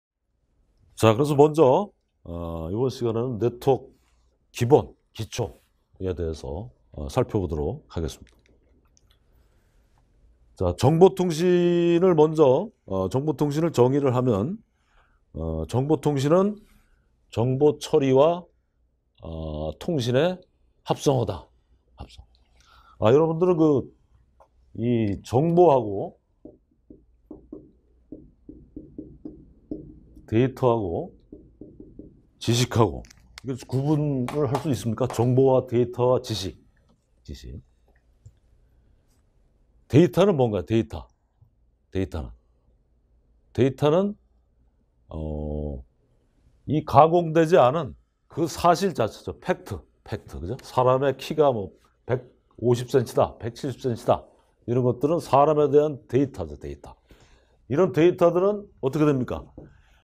[4. 네트워크 (정보처리기술사 양성 과정)] Ⅰ. 네트워크 기본(1)
(주) 인포레버컨설팅에서 주관하여 진행된 정보처리기술사 양성 과정 중
"네트워크" 강의입니다.

Ⅰ. 네트워크 기본(1)

네트워크 기초
네트워크 정의 및 구성요소